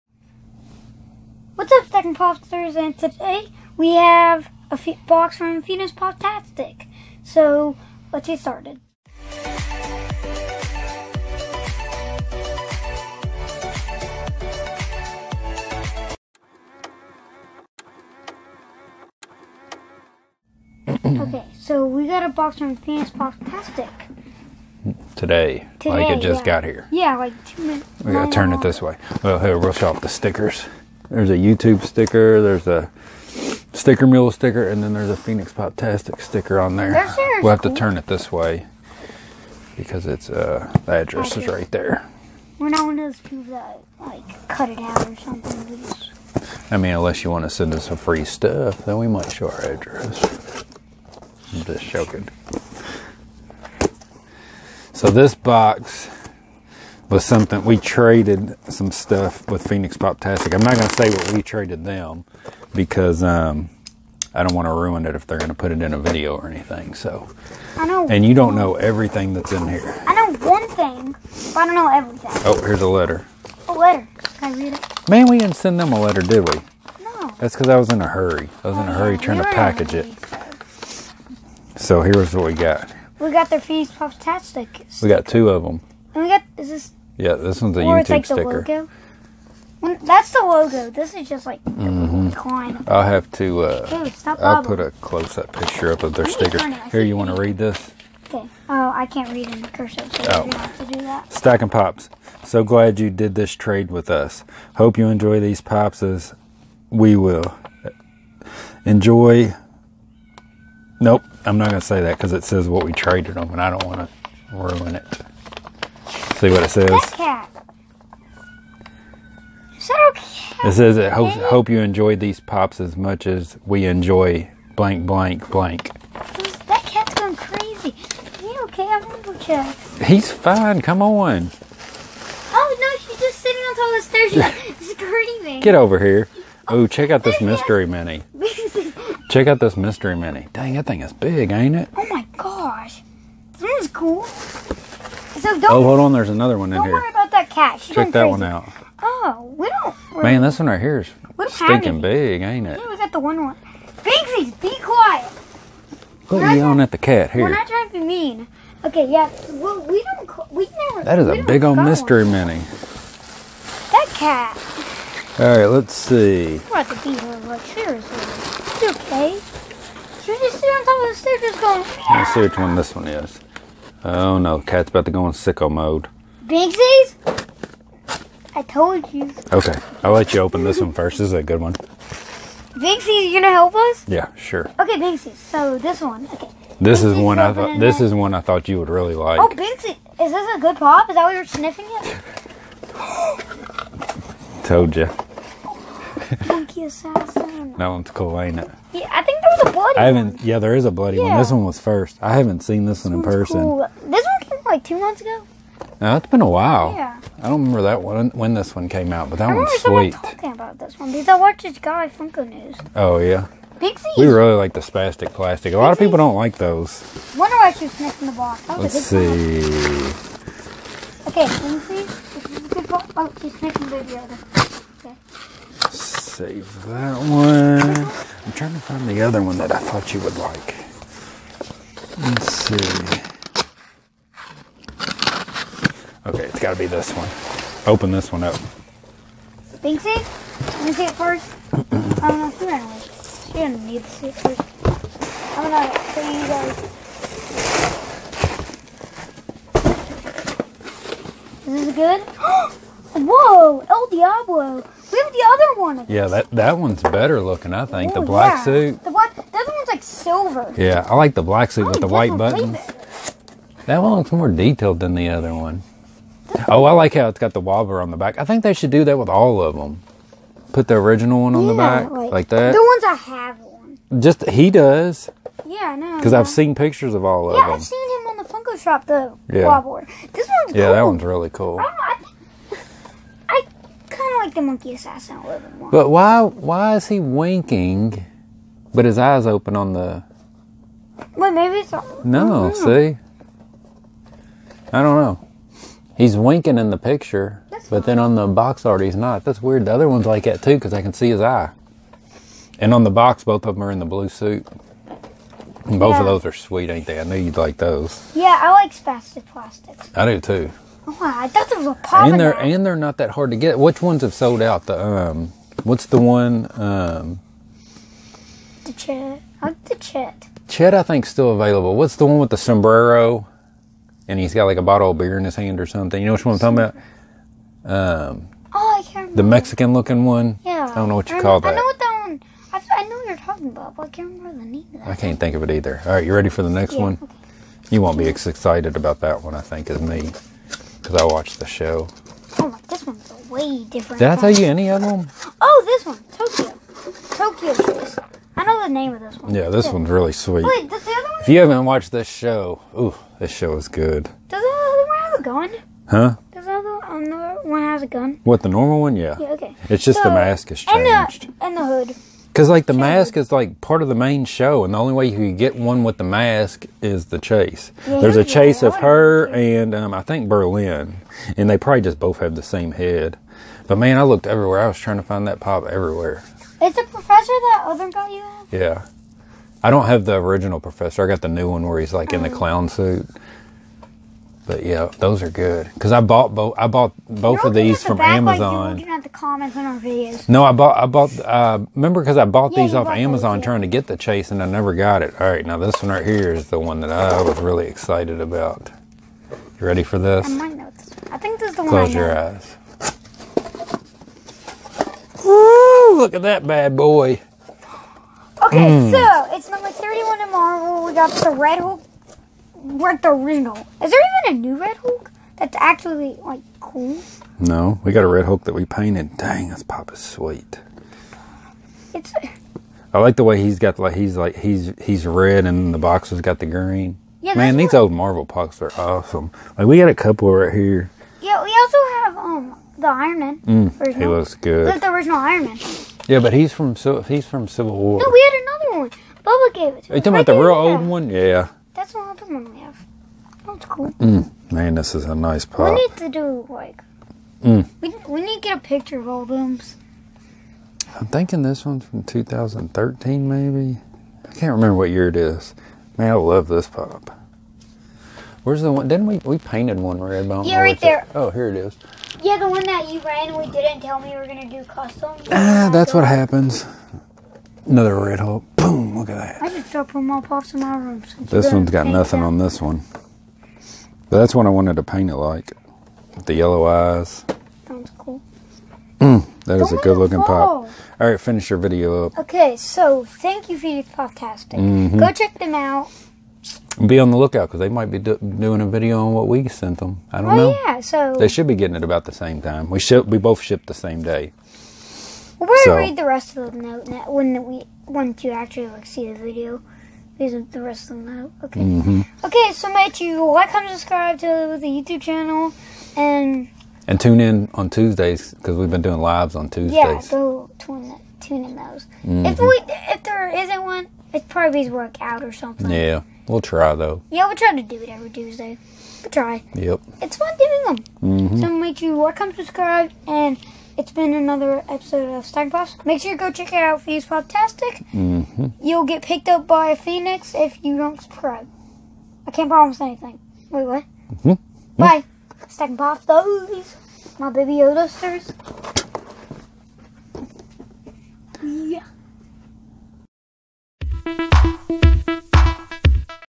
Awesome Trades From PhoenixPOPtastic GRAIL ALERT!!!
Take a look at a few Funko Pops we got from Phoenix POPtastic today in a trade we did with them. Lots of awesome stuff, including 2 personal GRAILS!!!